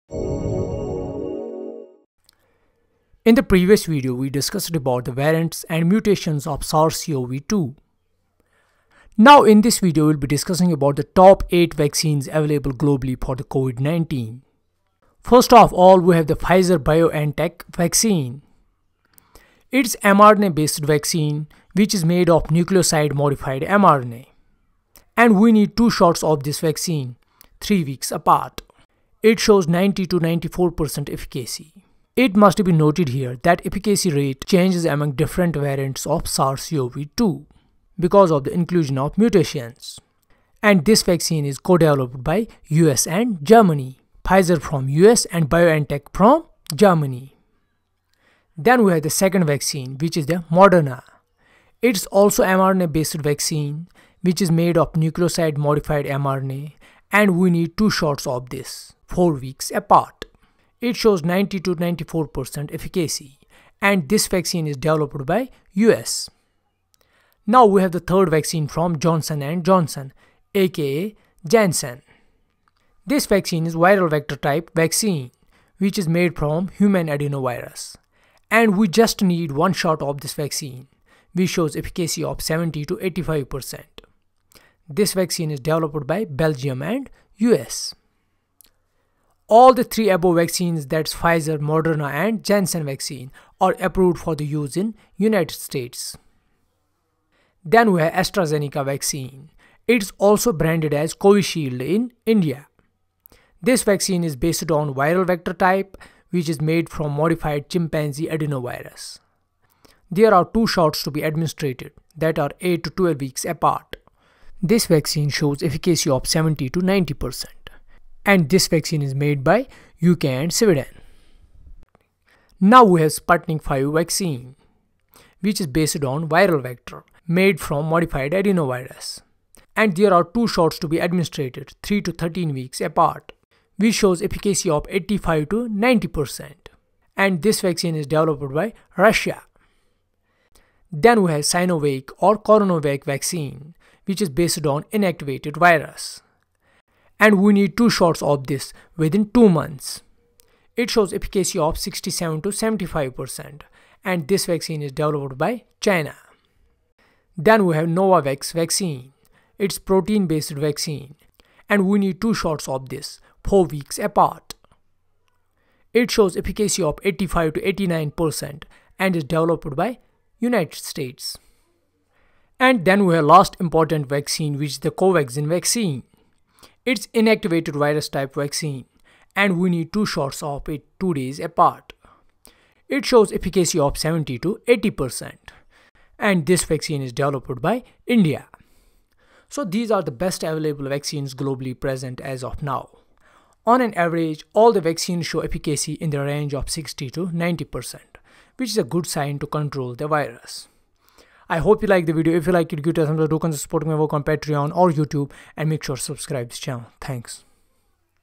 Top 8 Vaccines for Covid-19 | Comparison
In this video we have discussed about the Vaccines globally present for the Covid-19.
These 8 vaccines shows the efficacy of 60-90 % depending upon the variant we are targeting. The top 8 vaccines that we have currently present includes the :
Pfizer BioNtech
Moderna
Johnson and Johnson
Astrazeneca /Covisheild
Sputnik V
Novavax
Covaxin
Sinovac/Coronavac